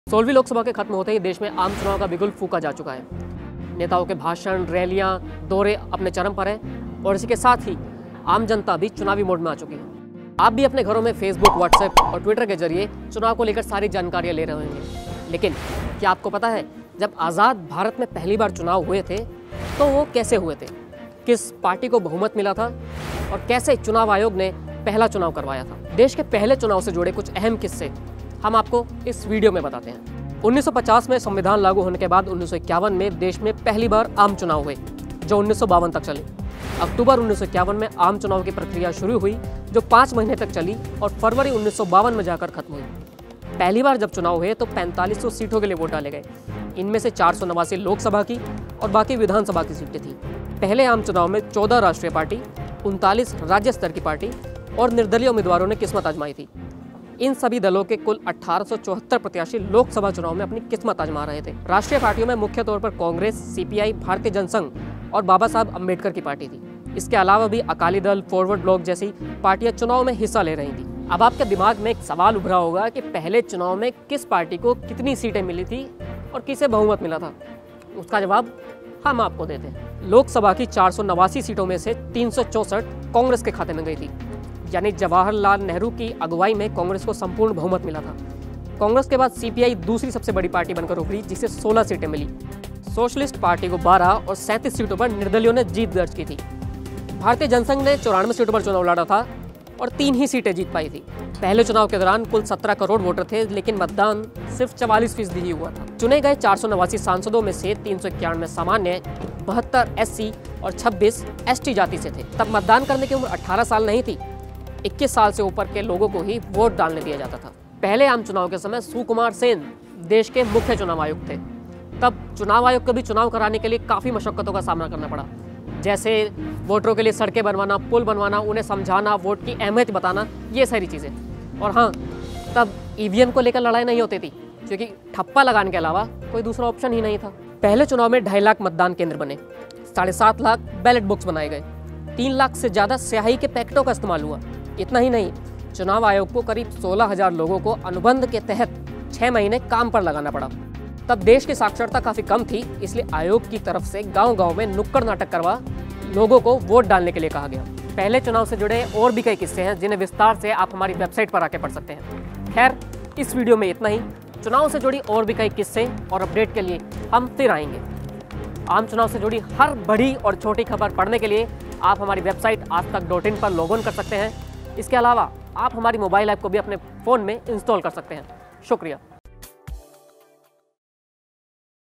देश में कैसे हुआ था पहला लोकसभा चुनाव, जानें दिलचस्प किस्से
लोकसभा चुनाव को लेकर देश में चुनावी माहौल चरम पर है. Aajtak.in इसी कड़ी में आपको चुनाव से जुड़े कई किस्सों के बारे में बताएगा. इस वीडियो में आप देखिए देश में जब आजादी के बाद पहली बार लोकसभा चुनाव हुए थे, तो किस तरह हुए थे और किस पार्टी को बहुमत मिला था. पहले लोकसभा चुनाव से जुड़े और भी कई किस्से इस वीडियो में देखें...